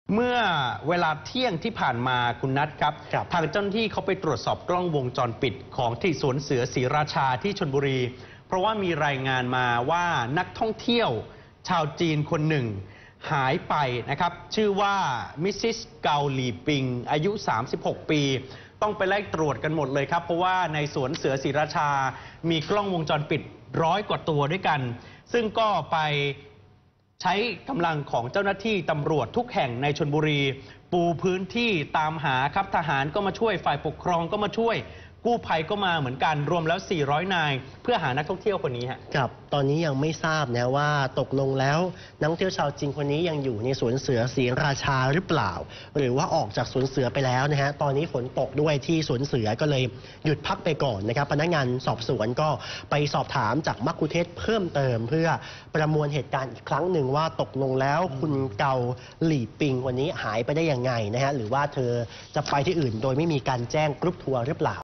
News ปูพรมหานักท่องเที่ยว ไล่ดูวงจรปิดสวนเสือศรีราชาหาเบาะแส
พิกัดข่าวค่ำทั่วไทย : ปูพรมเดินหน้าหานักท่องเที่ยว ไล่ดูกล้องวงจรปิดสวนเสือศรีราชาหาเบาะแส "NOW26" 12-9-59